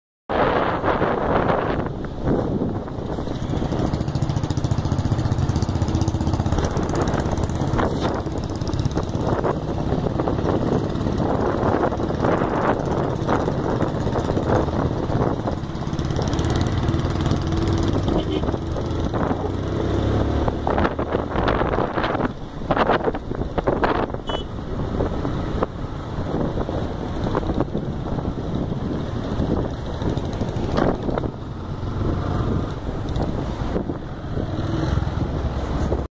A Ride through Parvati Valley on Yamaha R15 2.0, Royal Enfield Thunderbird and Hero Karizma
This video was captured during our trip to Kasol.